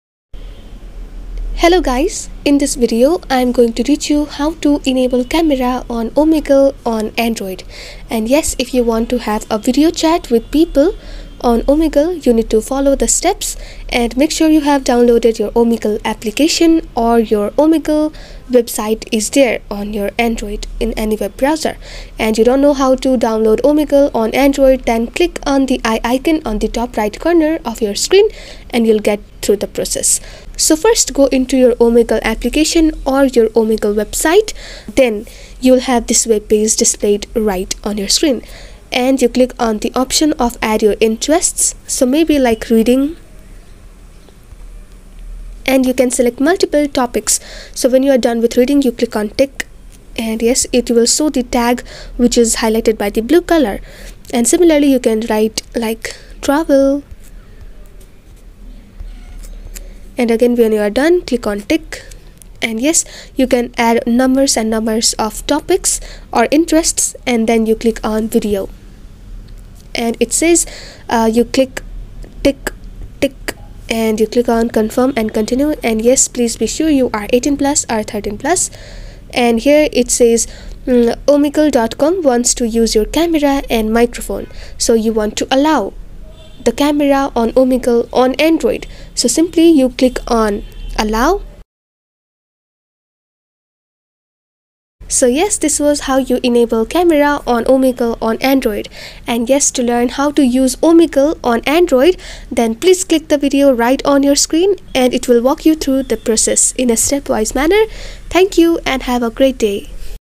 How To Enable Camera On Omegle On Android
In this video, we will be showing you how to enable your camera on Omegle while using an Android device. This is a very simple process that only takes a few minutes to complete.

00:05 How To Enable Camera On Omegle On Android
00:30 Open Omegle Webiste Or Applicaton
01:00 Enable Camera On Omegle On Android